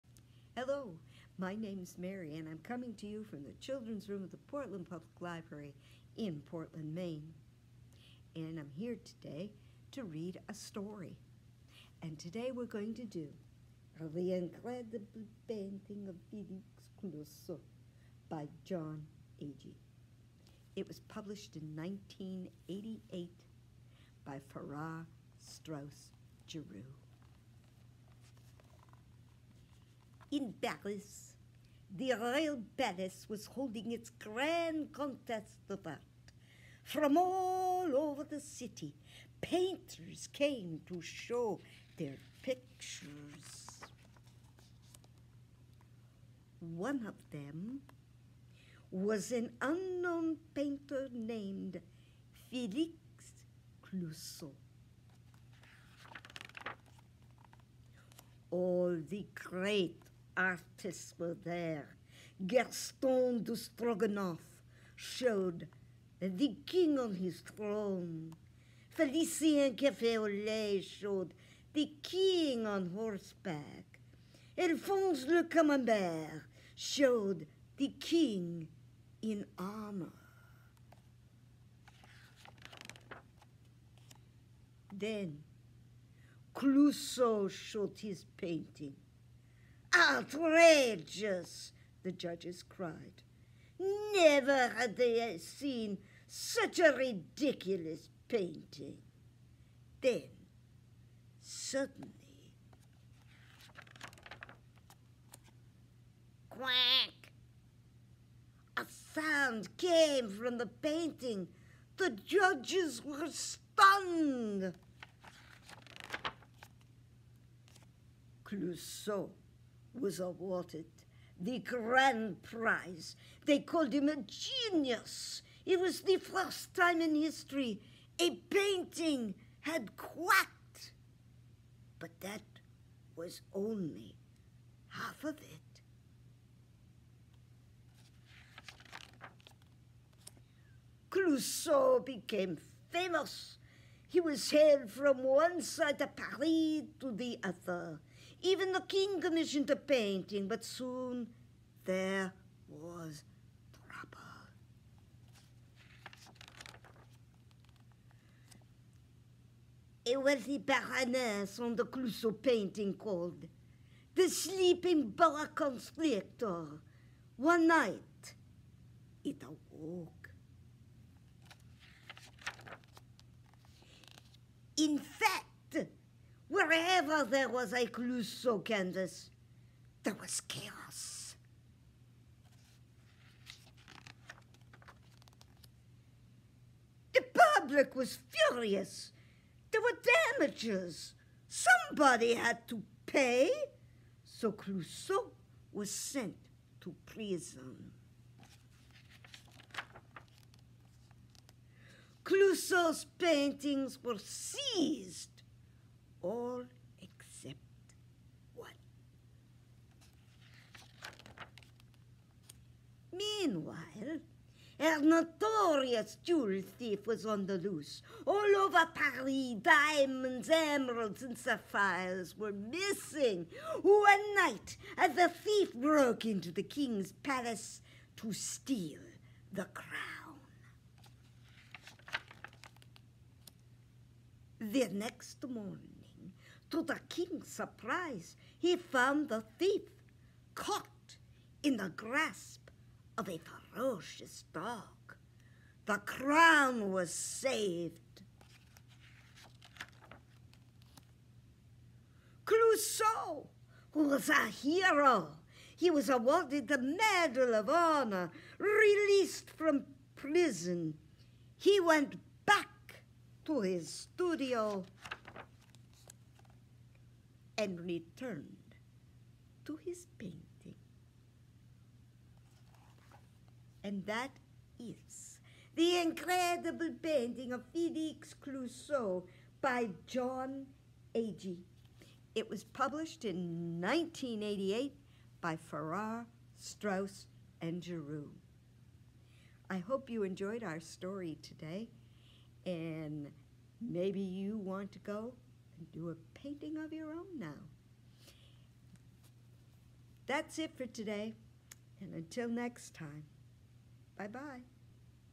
The Incredible Painting of Felix Clousseau read aloud
Join Mary of Portland Public Library as she reads The Incredible Painting of Felix Clousseau, written and illustrated by Jon Agee. This book was published by Square Fish. These read alouds premiere each Wednesday at 10:30 am. Relax and listen along!